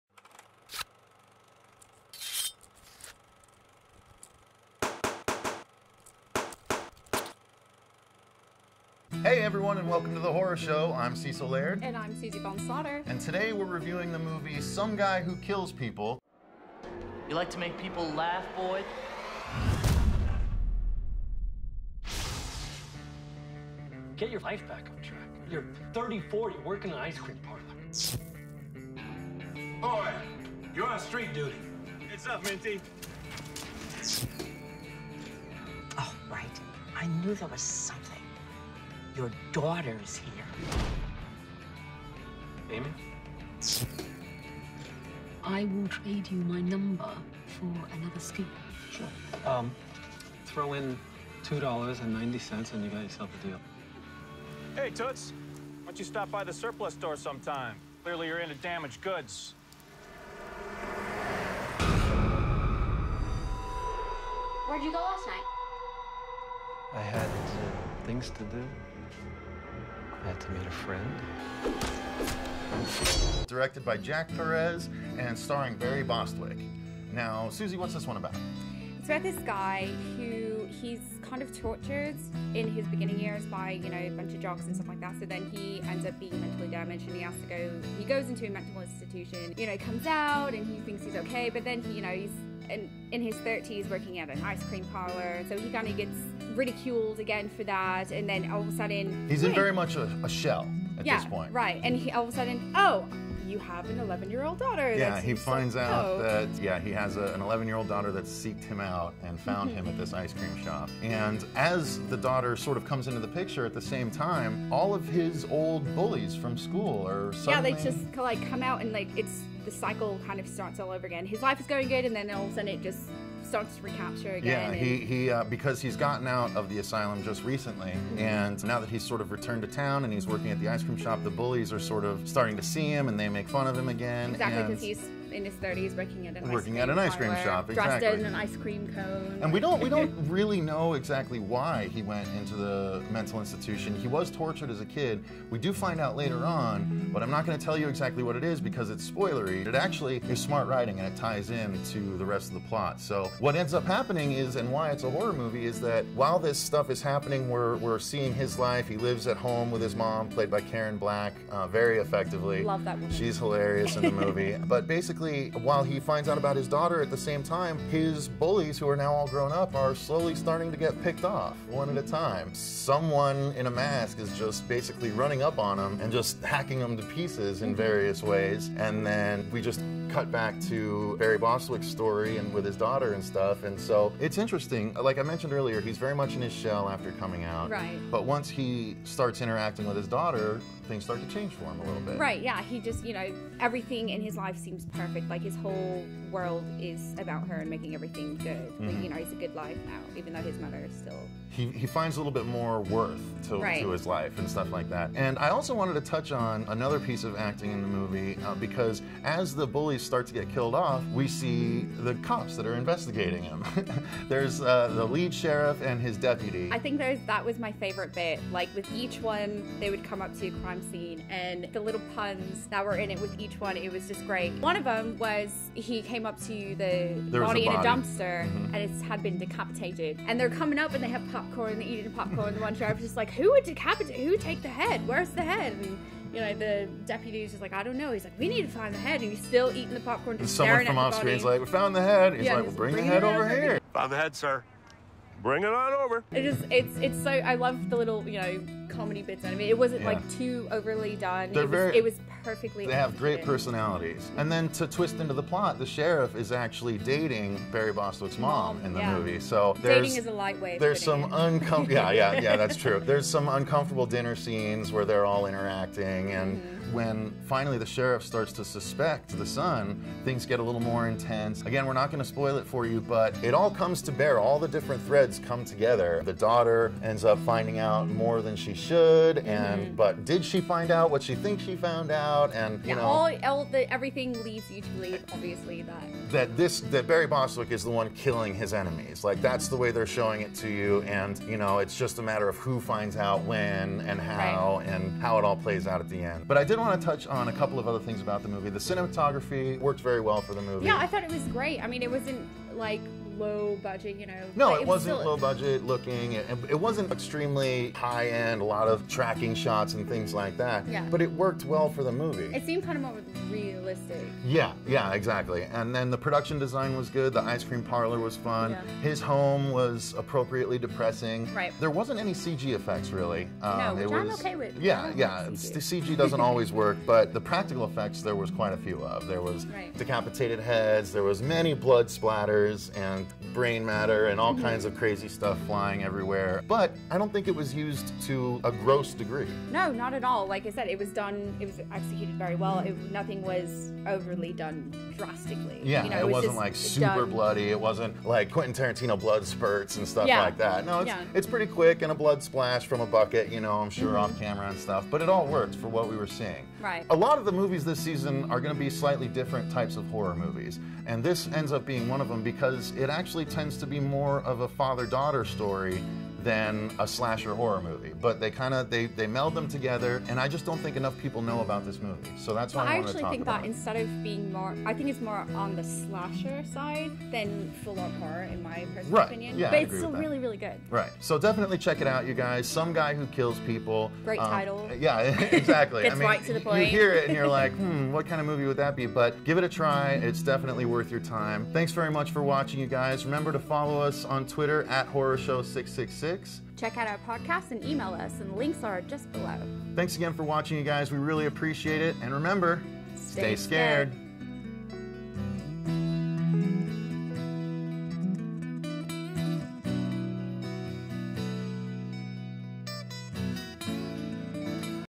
"Some Guy Who Kills People" Review - The Horror Show
This is our regular review of the movie "Some Guy Who Kills People". Be sure to check out the spoiler review as well!

**CORRECTION: The main character is played by Kevin Corrigan and the Sheriff is played by Barry Bostwick! Whew! It was a long day of filming...

Hosts: Cecil Laird & Susie Von Slaughter
Produced & Edited by Cecil Laird

Produced & Equipment/Studio by Steve Wargo & SWATHD.
Opening & Closing Logo Designs by Rolo Ledesma & Kristopher Anderson.